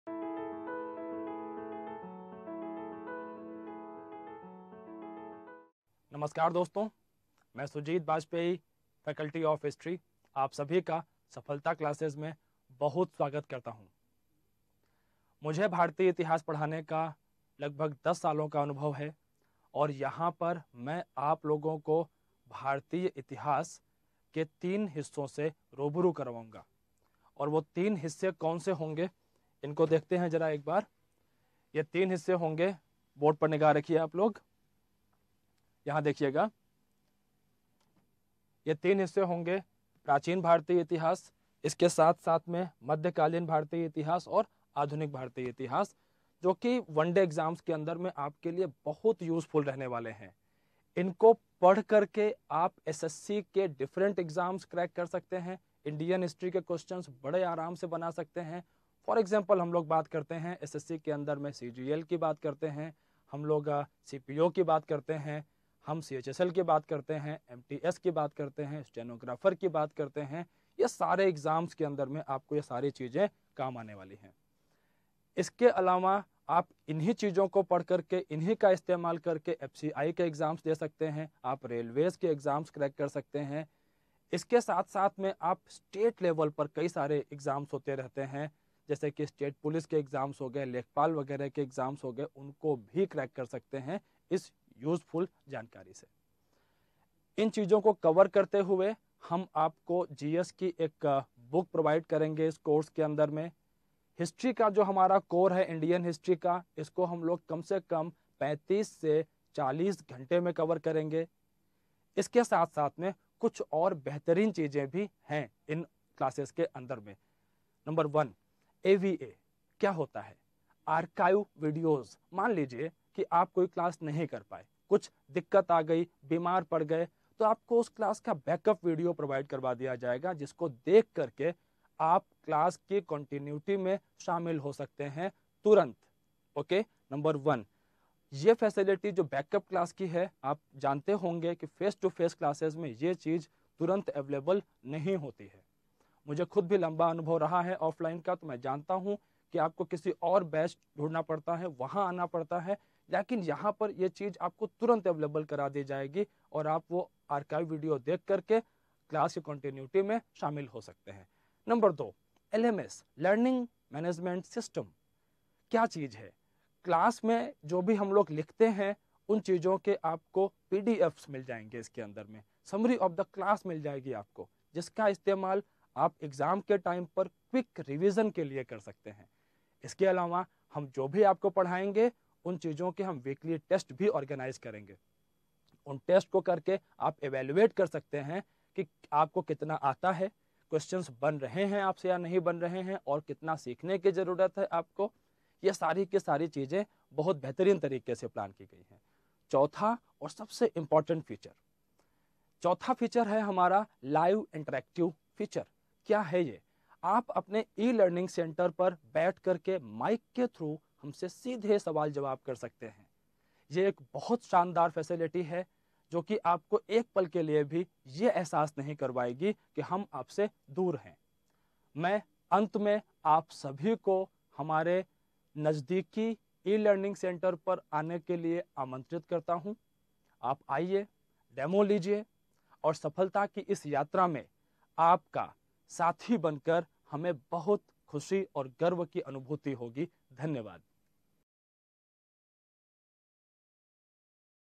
SSC Exam Preparation Free by Sujit Bajpai Sir at Safalta Class Foundation Course
SSC Exam Preparation Free by Sujit Bajpai Sir at Safalta Class Foundation Course:- भारत में पहली बार Safalta Class लेकर आ रहा है SSC और अन्य सरकारी नौकरी की तैयारी के लिए फ्री फाउंडेशन कोर्स

Hope you are liking the session.

For Free session Updates & Schedule Join our telegram Group